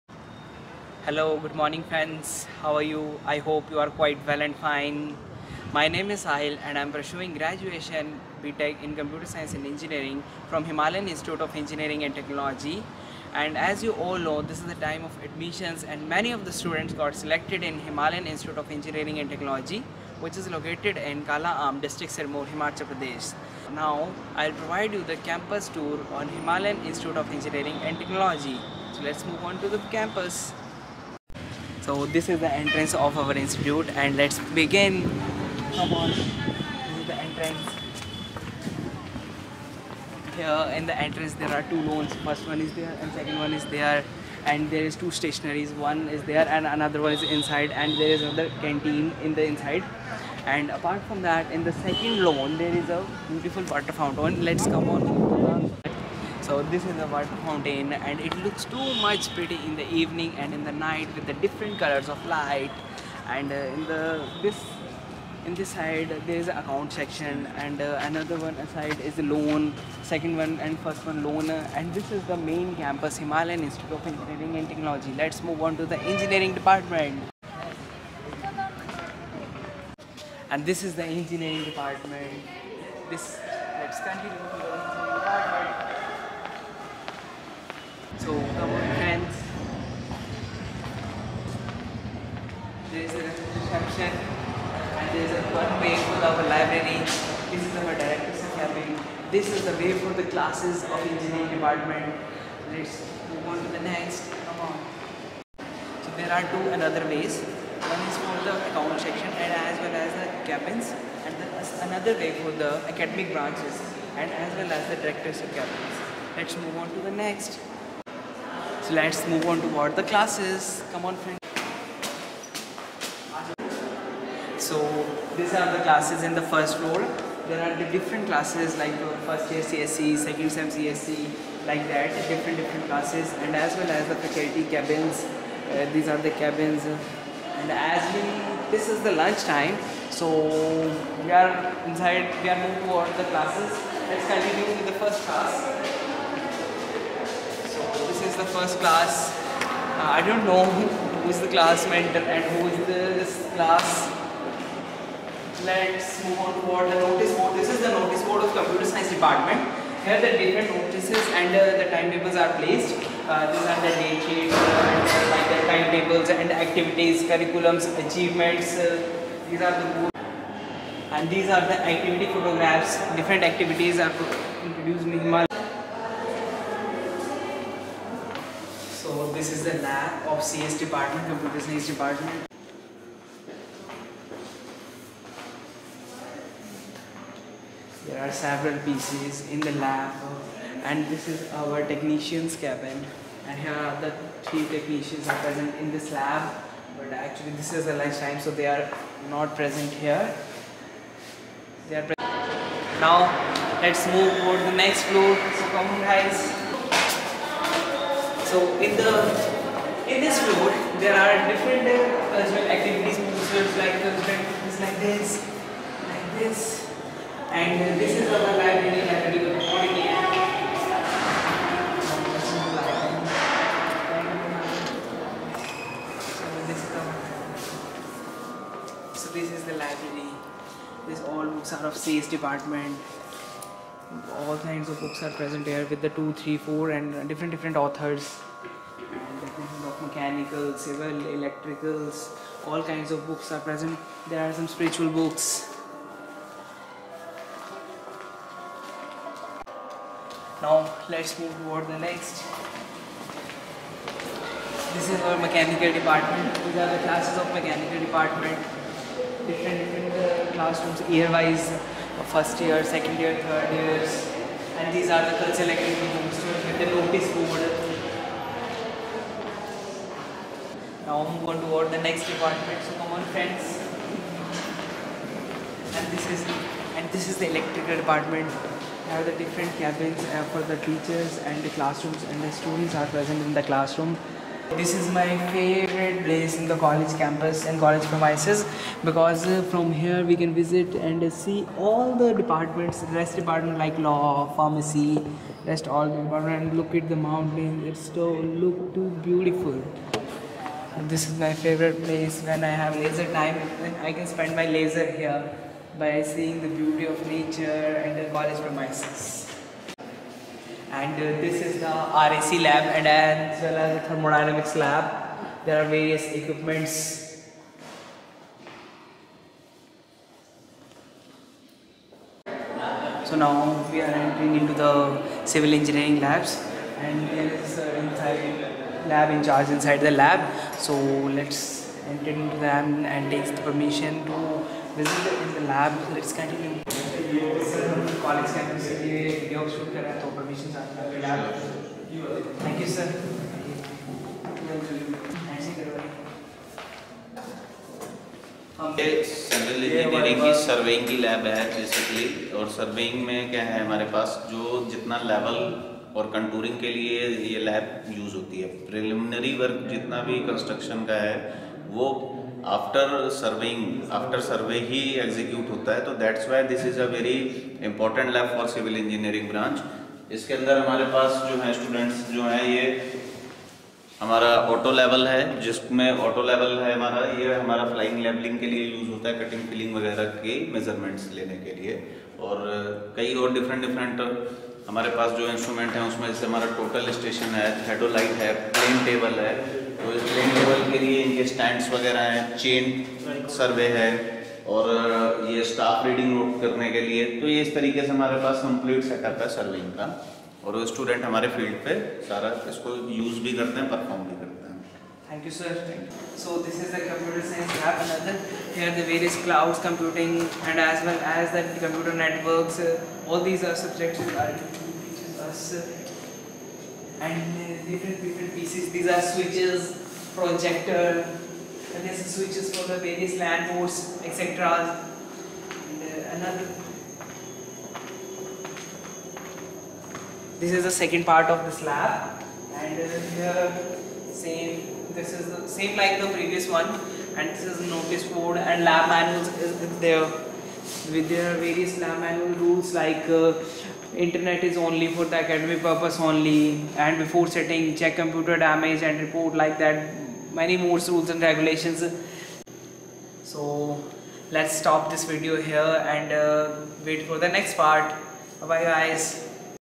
Himalayan Institute of Engineering & Technology | Campus Visit | HGPI
Himalayan Institute of Engineering & Technology is promoted and fully funded by the Maa Saraswati Educational Trust keeping in mind the needs of students of Himachal Pradesh. Himalayan Institute of Engg. & Technology was started so that students of 10+2 Non Medical stream can avail the opportunity to enter into the Engineering profession.